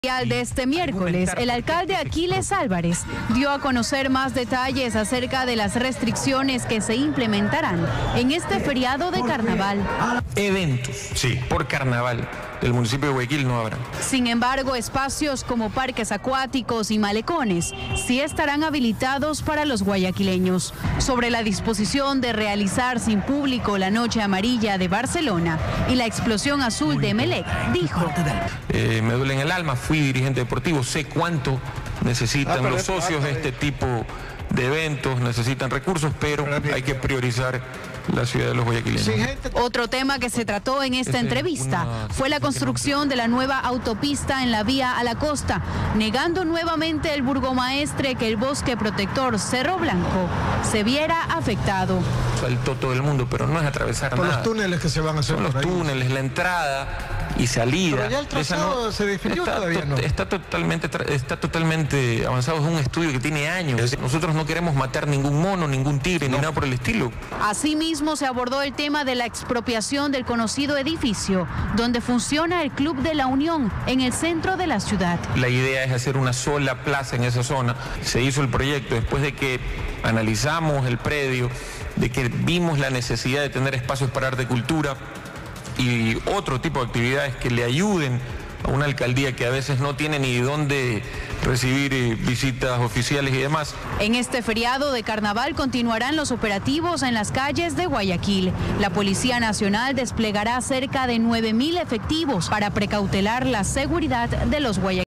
El alcalde Aquiles Álvarez anunció que todos los eventos de carnaval han sido cancelados
El alcalde Aquiles Álvarez informó que en Guayaquil todos los eventos municipales de carnaval, han sido cancelados. Sin embargo, parques acuáticos si estarán operativos para las familias. Por: María Mercedes Pesantes